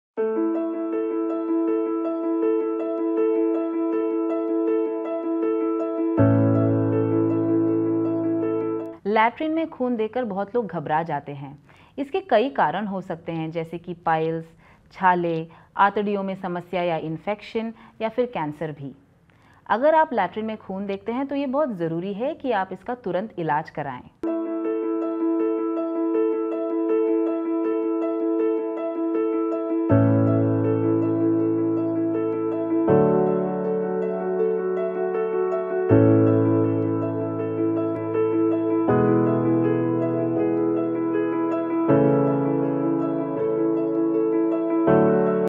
लैट्रीन में खून आने का क्या कारण है?
डॉ. गौरी कुलकर्णी,एम.बी.बी.एस, एम.आर.सी.जी.पी [आइ.एन.टी], बी.एस.आई.सी (बी.ए.सी.पी)
सामान्य चिकित्सक

लैटरिंग में खून देख कर बहुत लोग घबरा जाते हैं |

इसके कई कारण हो सकते हैं जैसे कि - बवासीर, छाले, आँतड़ियों में समस्या या इंफैक्शन या फिर कैंसर भी |

अगर आप लैटरिंग में खून देखते हैं तो यह बहुत ज़रूरी है की आप इसका इलाज करवाएं |

अगर आपको ऐसी कोई भी समस्या है तो आप डॉक्सऐप पर डॉक्टर की सलाह ले सकते हैं |

लैट्रीन में खून आने के कारण :

        बवासीर
         छाले
        आँतड़ियों में समस्या
        कैंसर

More info.

मल में खून आना या गहरे काले रंग का मल आना एक मेडिकल स्थिति है, जिसे मेडिकल भाषा में मलीना के नाम से जाना जाता है। मल में खून आना खुद कोई रोग नहीं है, लेकिन यह कई अंदरूनी शारीरिक समस्याओं का संकेत हो सकता है। दूसरे शब्दों में मलीना यानि मल में खून आना सिर्फ एक लक्षण होता है। मल का रंग काला पड़ने की मुख्य वजह यह भी हो सकती है कि उसमें काफी मात्रा में खून मिला हो।

मल में खून आना पाचन तंत्र में खून बहने का परिणाम होता है। खून, विशेष रूप से पेट, इसोफेगस या छोटी आँत के ऊपरी हिस्से से बहता है। पाचन तंत्र थोड़े बहुत खून की मात्रा को खाद्य पदार्थों की तरह पाचन क्रिया में शामिल कर देता है, जिस कारण से मल काला दिखने लगता है।

अगर मल में अधिक खून आ रहा हो तो यह एक मेडिकल इमर्जेंसी है। यदि मल काला तथा टार जैसा आ रहा है, तो इसकी जांच हमेशा एक डॉक्टर द्वारा ही की जानी चाहिए।

कब्ज़ और सूखे मल द्वारा घाव होने पर बवासीर होने के कारण
फिस्चुला, फिशर, गुदा या बड़ी आँत में पॉलीप्स मल के साथ ब्लड आने के गंभीर कारण हैं
गुदा का कैंसर
बड़ी आँत का कैंसर
आँत में संक्रमण जैसे कोलाइटिस
अल्सरेटिव कोलाइटिस (आंत की सूजन)
इनके अलावा किसी खाद्य पदार्थ से एलर्जी और खून से सम्बंधित रोग के कारण भी ब्लड आने की समस्या हो सकती है|

अगर आप मल में खून आने की समस्या से परेशान हैं तो तुरंत हमारे विशेषज्ञ से बात करें